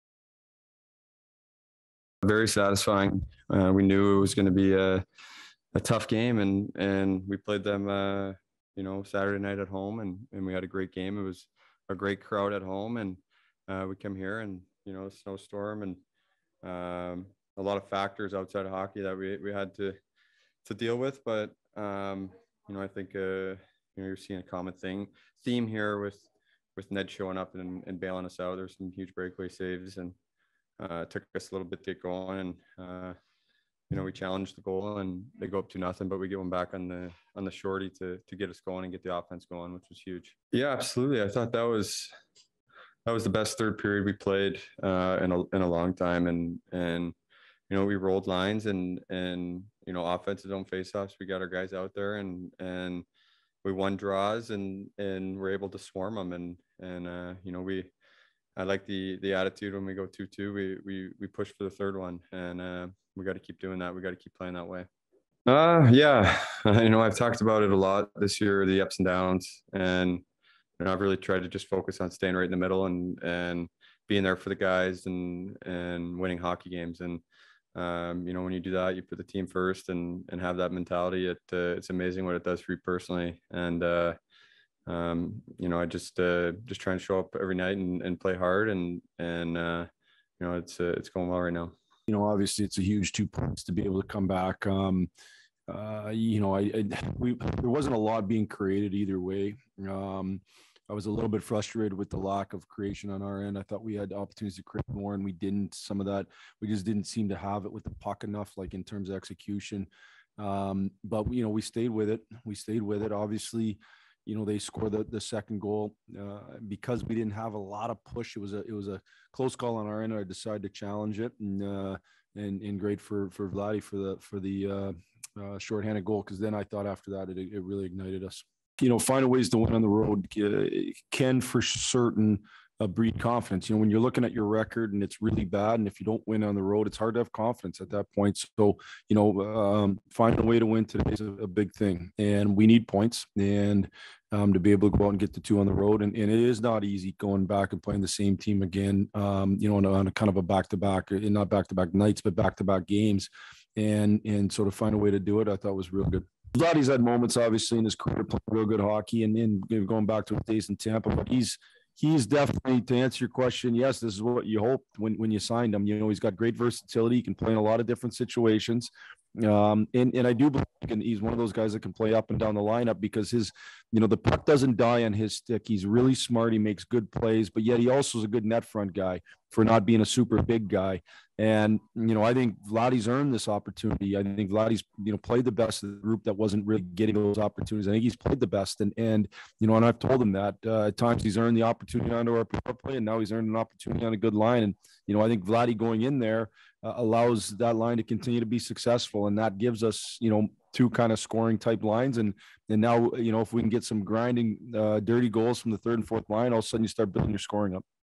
Detroit Red Wings | Meijer Post Game Comments | DET vs BUF - 1/17/22
Dylan Larkin and Jeff Blashill speak with the media following the Red Wings 3-2 overtime win over the Buffalo Sabres at The KeyBank Center.

Presented by Meijer